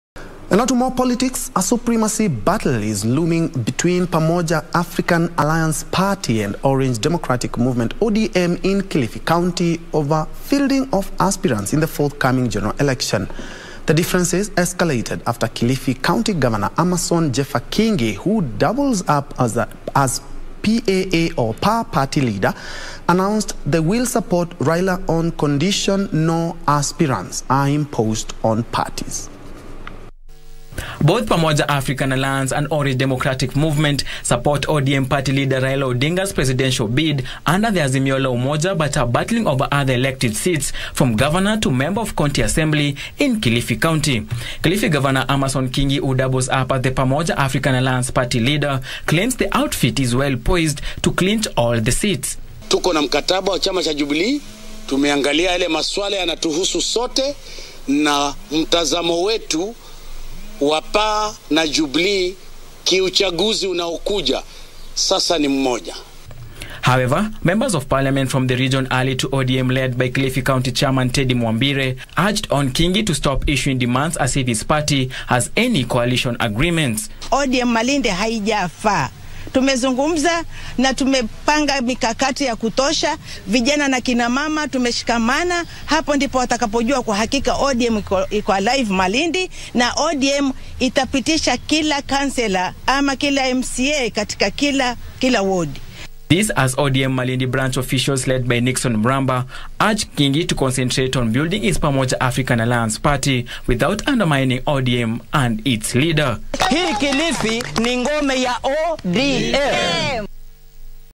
Supremacy battle looms between PAA and ODM party in Kilifi County
A supremacy battle is looming between Pamoja African Alliance party and Orange Democratic Movement ODM in Kilifi County over fielding of aspirants in the forthcoming general election. The differences escalated after Kilifi County  Governor Amason Kingi who doubles up as the PAA party leader announced they will support Raila on condition no aspirants are imposed on parties.